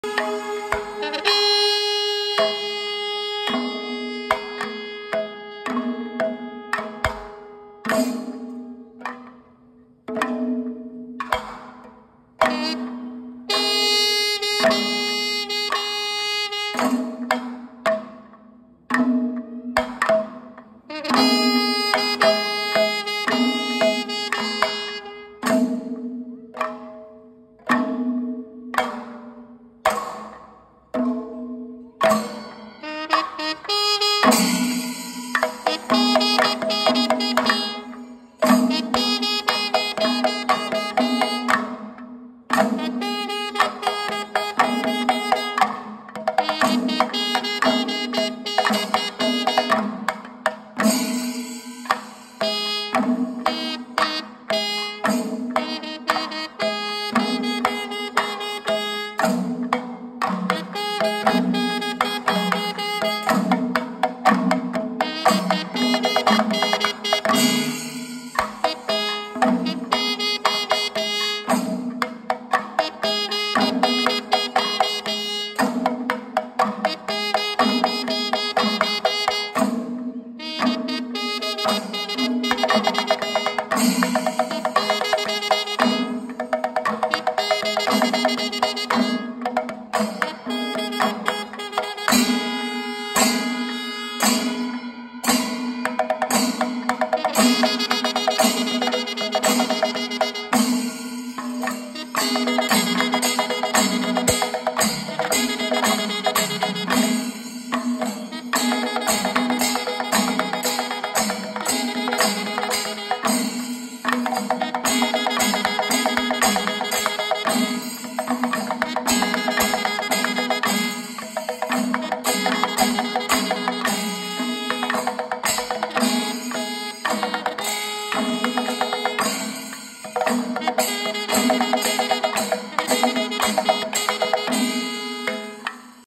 Panchavadyam Pathikaalam mugham by Ottapalam Hari at Ettumanoor
Panchavadyam lead by Chottanikkara vijayan marar, Cherpulasery sivan and party